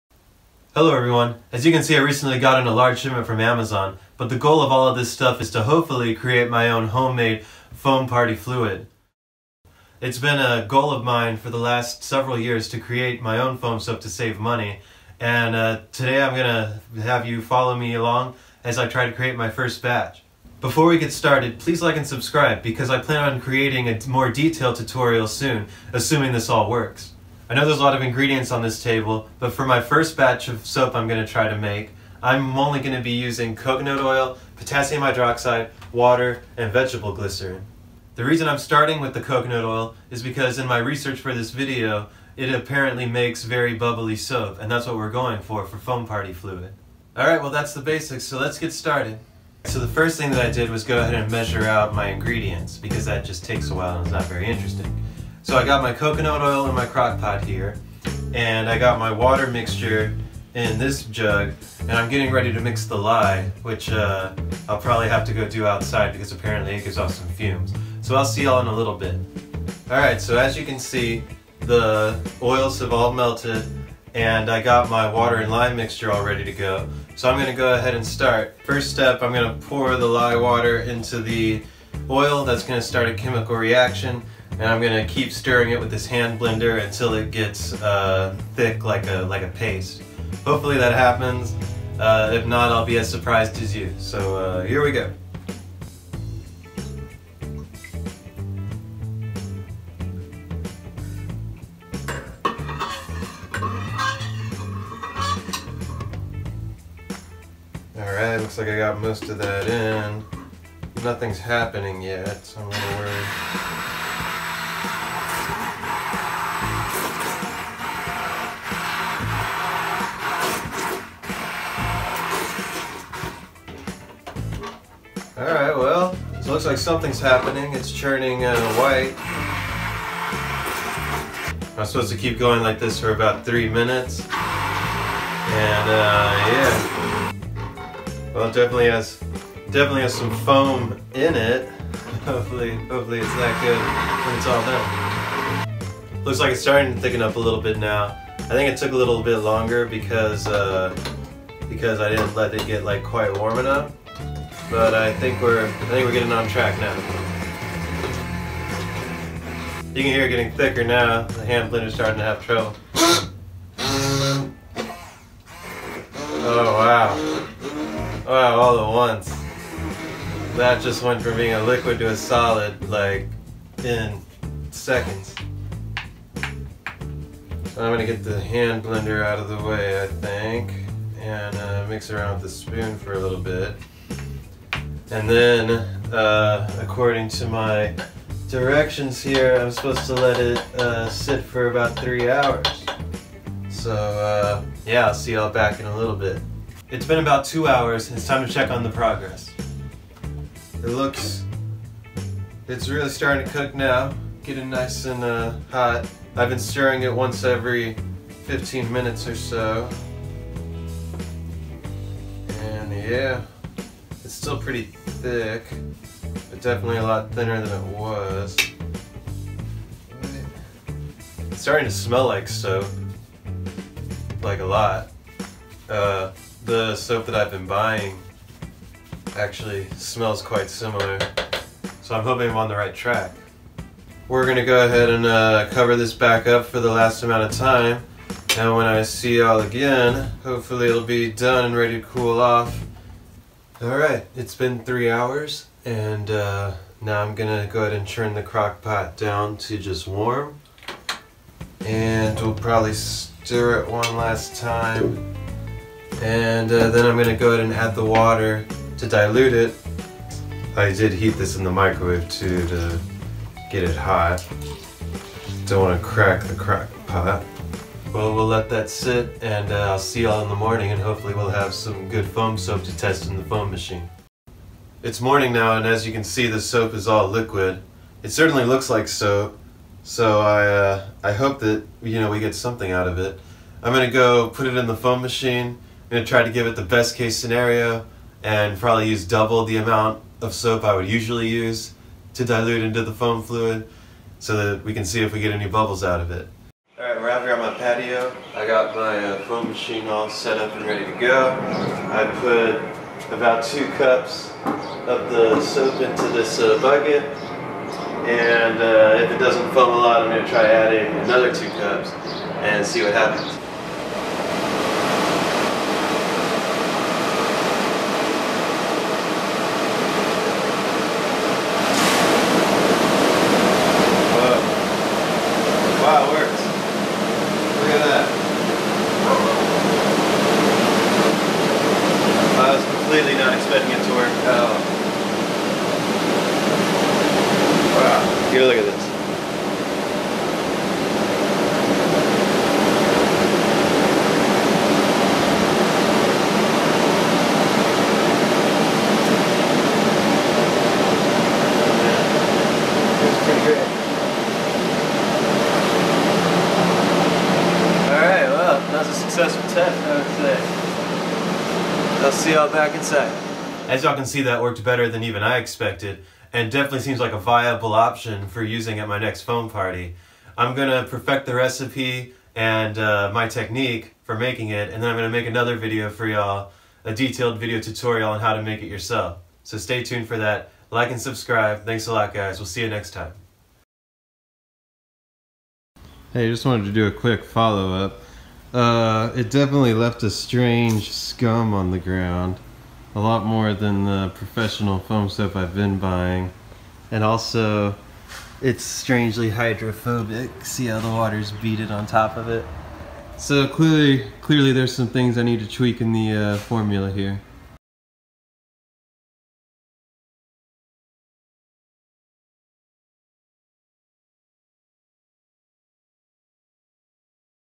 The Quest For DIY Foam Machine Soap
For years I’ve been buying expensive foam machine soap for my parties now fallow alongside me for my fist attempt at creating home made (DIY) foam soap.

This is the first video in what I hope will be a series as I try to prefect the recipe. I plan on posting a detailed tutorial for you all when I land on a good formula.

I am a total beginner at soap making so If you have any ideas on making good soap for foam parties your advice would be greatly appreciated.